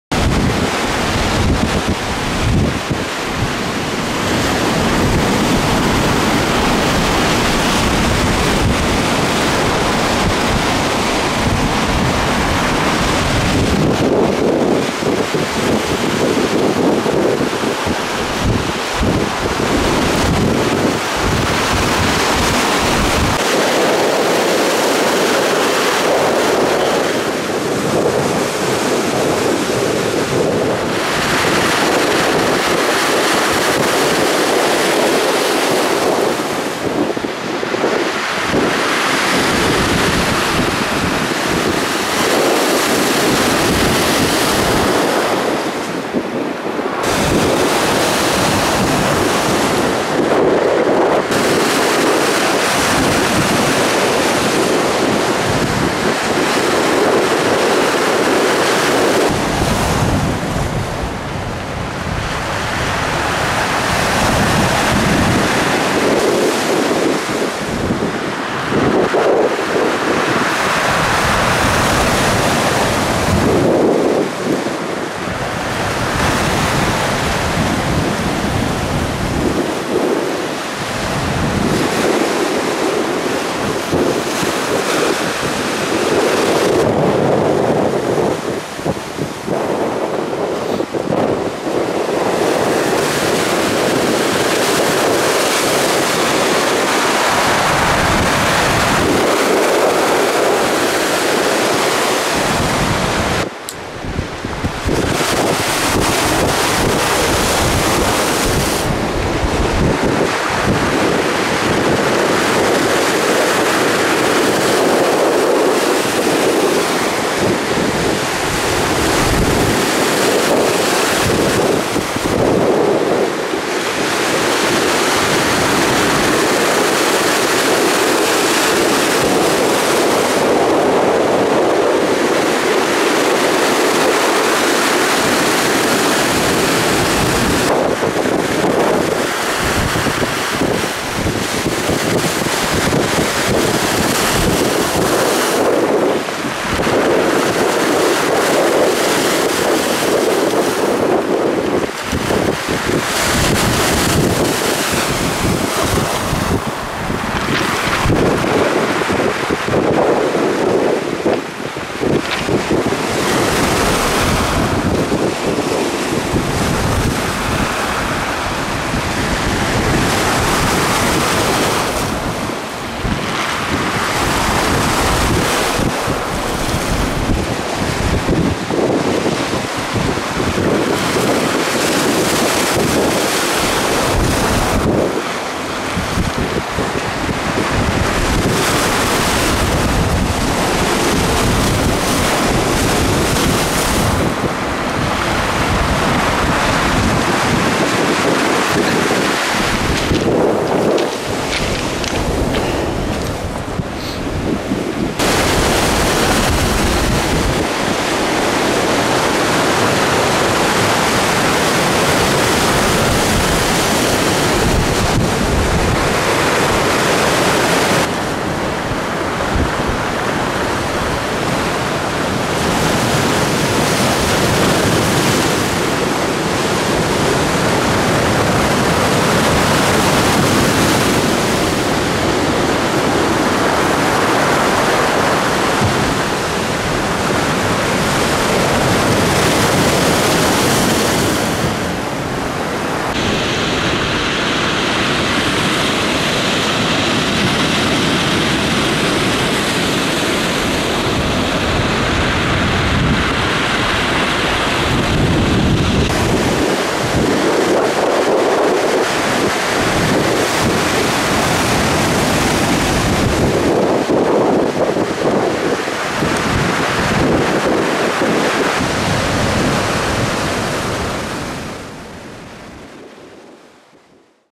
Финский залив, Эстония 07/12/2015, шторм
Шторм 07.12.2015, которым всех напугали и присвоили оранжевый уровень )))))
Съёмки сделаны в г. Силламяэ